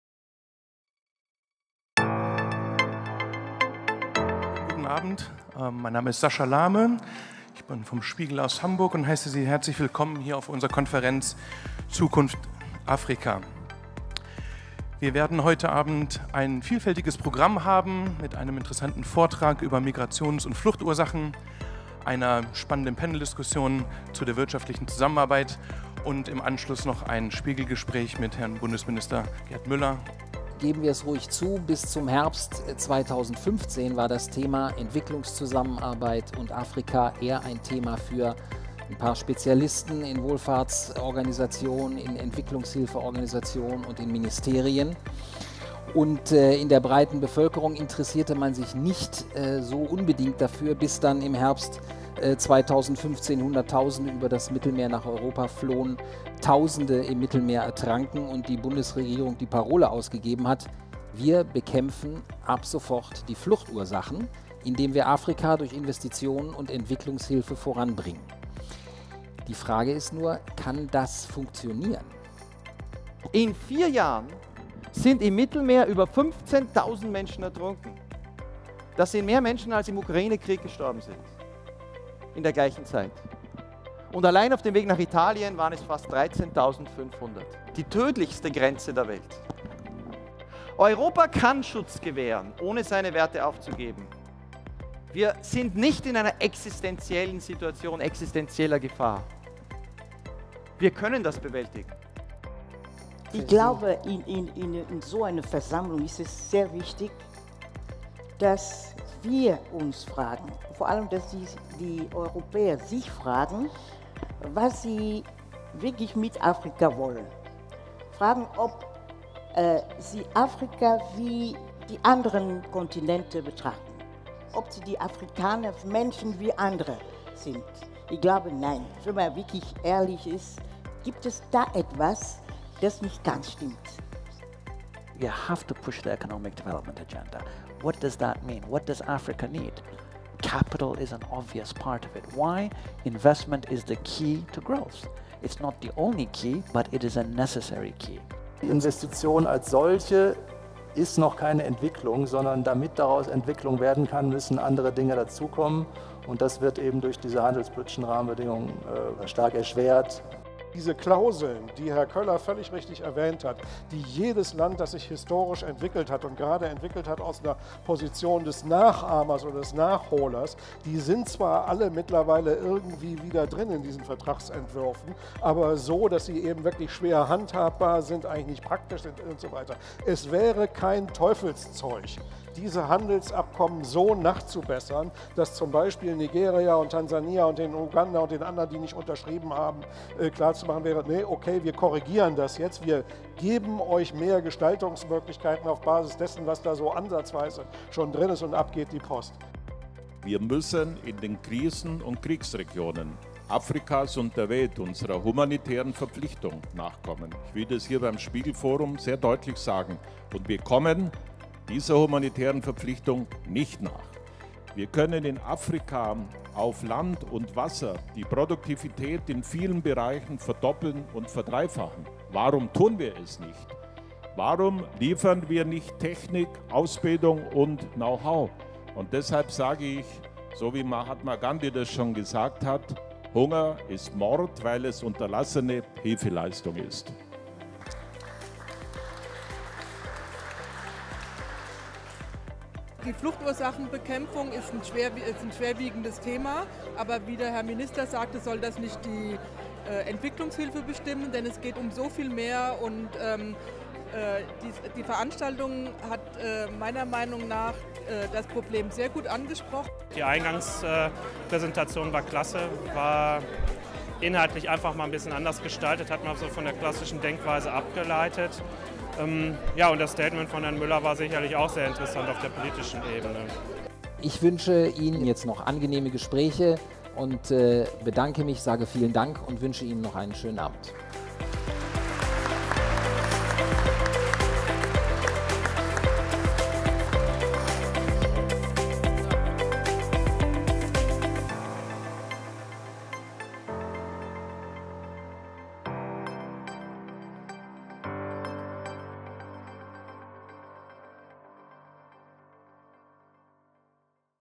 ZUKUNFT AFRIKA: Wie kann Entwicklungszusammenarbeit funktionieren?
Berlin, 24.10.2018

Afrika ist ein Kontinent der scheinbar widersprüchlichen Entwicklungen, zum einen liegen die Hälfte aller 20 weltweit am schnellsten wachsenden Volkswirtschaften hier. Das Potential an Arbeitskräften ist riesig. Andererseits fliehen die Menschen vor blutigen Konflikten, ist es nicht gelungen, islamistische Terrorgruppen zurückzudrängen. Staatliche Verwaltungsstrukturen leiden unter der grassierenden Korruption. Extreme Armut, Mütter- und Kindersterblichkeit sind nach wie vor in vielen afrikanischen Ländern hoch. Das rasante Bevölkerungswachstum frisst die Fortschritte durch wirtschaftliche Entwicklung auf.

Viele Herausforderungen, vor denen Europa heute steht, sind mit der Entwicklung afrikanischer Staaten verknüpft. Die gilt insbesondere für die anhaltenden Flucht- und Migrationskrisen. Diese Themen können nur kooperativ gemeistert werden – doch wie kann diese Zusammenarbeit am besten funktionieren?

Darüber sprechen:
- PROF. DR. HELMUT ASCHE, Wirtschaftswissenschaftler und Entwicklungsexperte, ifeas – Institut für Ethnologie und Afrikastudien, Johannes Gutenberg-Universität Mainz
- MARIE-ROGER BILOA, Chief Executive Director, The Africa International Media Group
- GERALD KNAUS, Vorsitzender, Europäische Stabilitätsinitiative (ESI)
- DR. THOMAS KÖLLER, Politikwissenschaftler, Verleger und Freihandelsexperte
- DR. GERD MÜLLER, Bundesminister für wirtschaftliche Zusammenarbeit und Entwicklung
- ADMASSU TADESSE, CEO, TDB – Trade and Development Bank

Die Moderatoren:
- NICOLA ABÉ, Redakteurin im Hauptstadtbüro, DER SPIEGEL
- FIONA EHLERS, Redakteurin im Auslandsressort, DER SPIEGEL
- MICHAEL SAUGA, Leiter Hauptstadtbüro, DER SPIEGEL